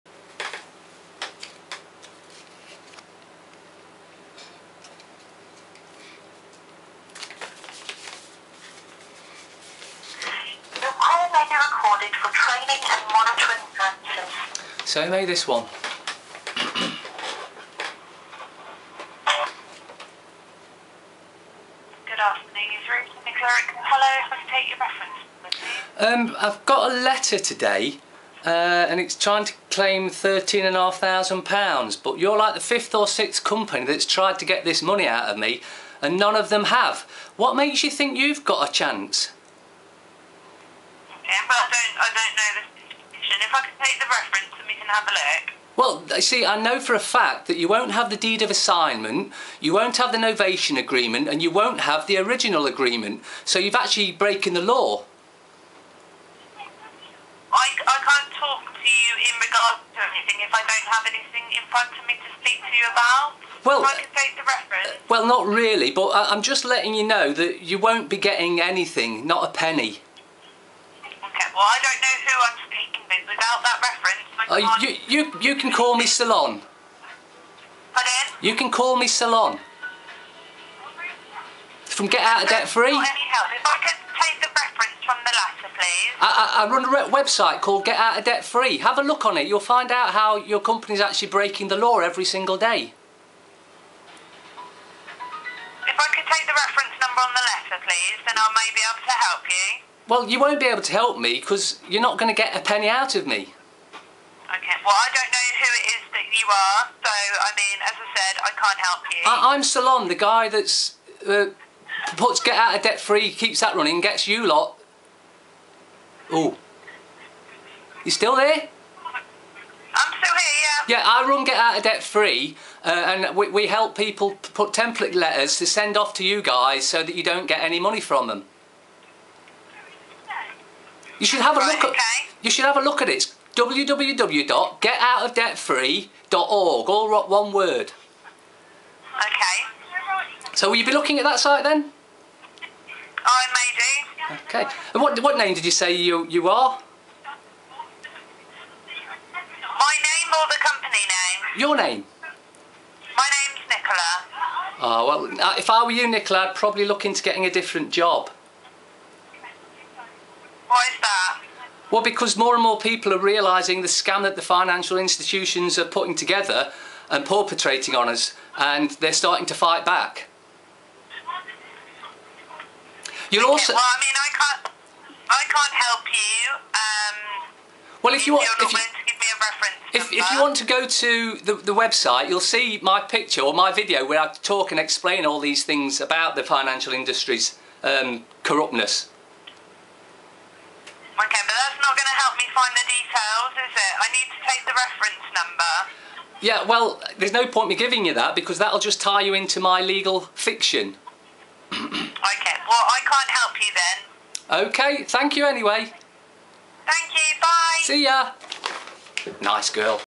mk rapid recoveries
got a letter from mk rapid recoveries so thought i would give them a call

nicola a very nice girl

perpetrate lol